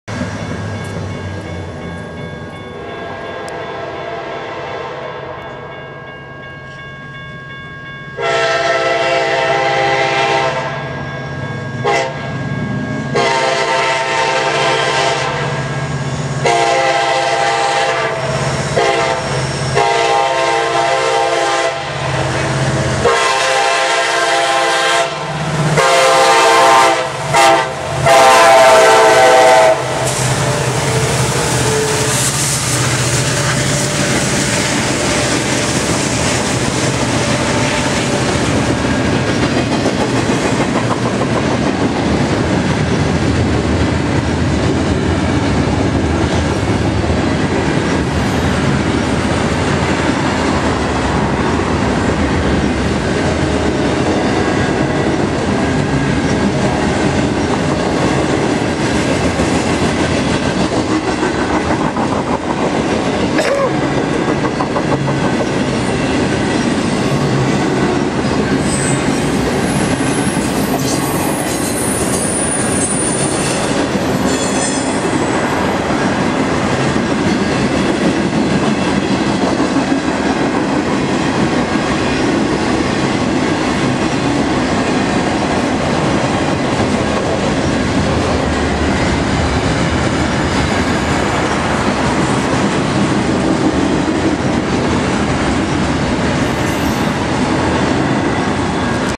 Norfolk Southern 23E WB Intermodal w/ KCS SD70ACe's in Austell,Ga 09-01-2012© (16x9)
11:39PM in Austell,Ga is NS 23E Westbound intermodal with KCS power! They were both EMD SD70ACe Southern Belle units but I couldn't see the road numbers...had the super loud G3 K5LA on it (like Jeff says uncalled for) lol.

KCS ? SD70ACe
KCS ? SD70ACe

Recorded by Gene and Tommy Hale on September 1, 2012